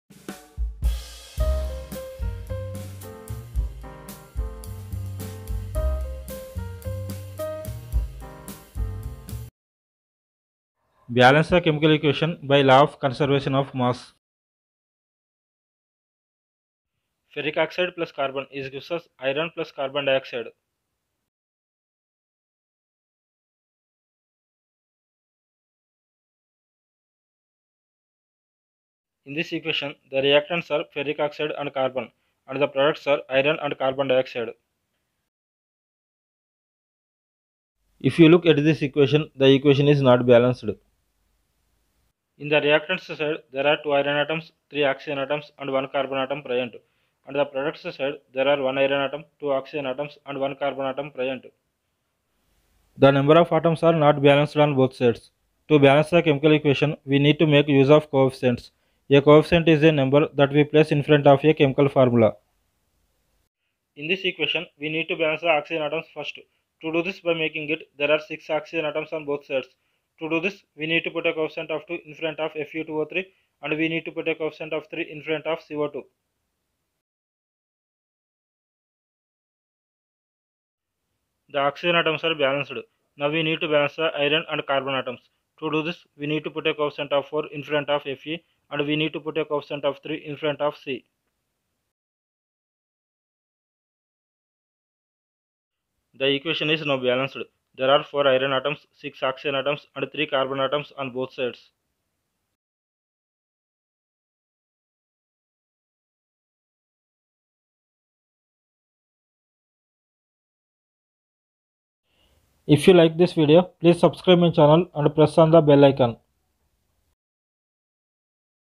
Fe2O3+C=Fe+CO2 balance the chemical equation @mydocumentary838. fe2o3+c=fe+co2 balance the equation
Fe2O3+C=Fe+CO2
fe2o3+c=fe+co2
ferric oxide+carbon=iron+carbon dioxide
ferric oxide and carbon are reacts to form iron and carbon dioxide